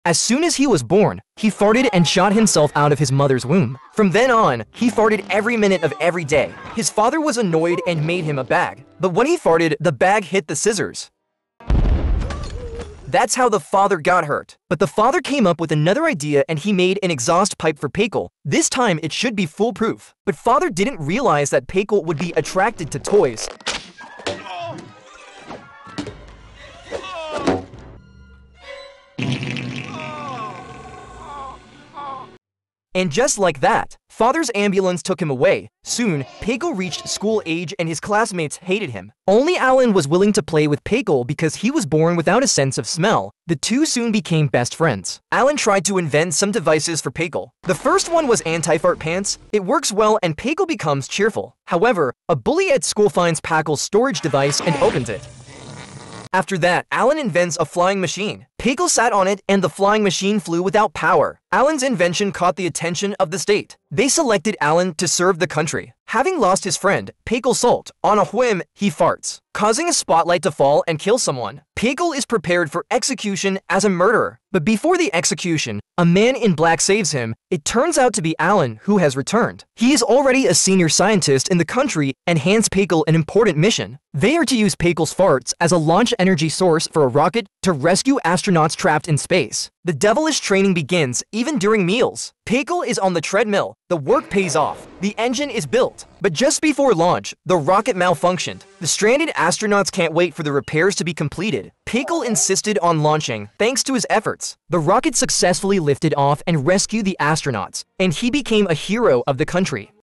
This kid was born to fart and went so far as to use his farts as power to launch a rocket!
Ever since he was born, Palka has had an extraordinary “farting ability”, a powerful energy that has hurt his father time and time again. Finally, after another fart attack from Palka, his father decides to live with his work and leaves the house for good. Since his father's departure, his sister Denise doesn't talk to him much and his mother spends her days in tears. As he grew older, Palka became more and more distressed, and instead of getting better, the farting problem got worse and worse, and the condition could happen at any time, regardless of the time or place. And every day, he has to endure the blank stares of teachers and the ridicule of his classmates, especially the bullying of Fatty Damon, which makes Parka fearful of school. Luckily, his best friend Alan is a child genius, and in order to help Palca solve this problem, Alan invents the Thunderbolt Pants, and from then on, he never has to worry about farting again. But even so, Palca can't regain his self-confidence.